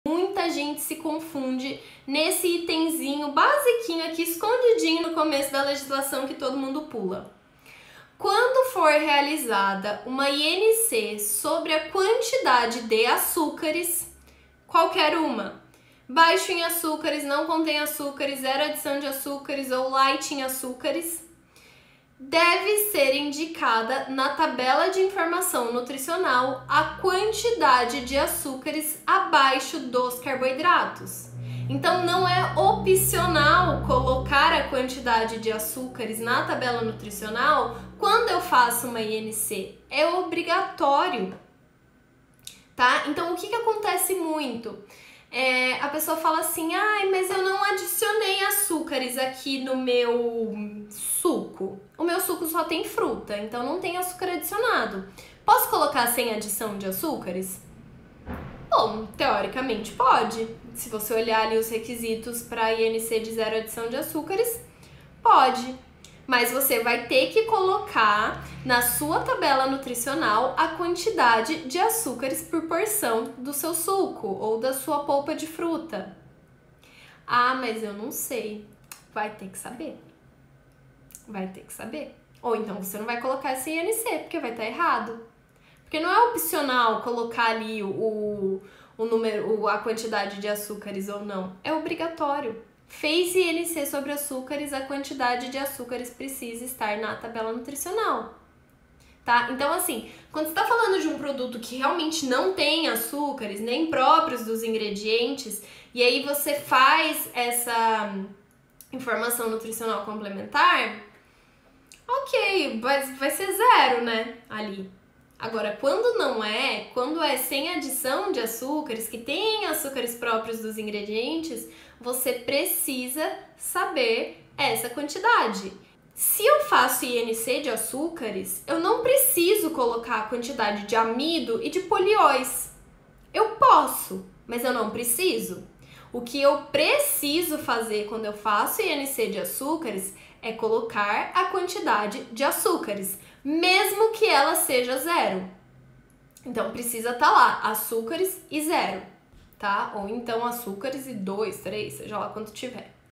Informação OBRIGATÓRIA quando se faz INC de açúcares
Muitos profissionais deixam de trazer esta informação na tabela nutricional, quando fazem INC de açúcares em um rótulo.

Entre para o nosso grupo vidaderotulagem no Telegram, sempre tem conteúdos novos para você conferir

Confira também conteúdos diários sobre rotulagem de alimentos e um pouco do que rola no meu dia a dia: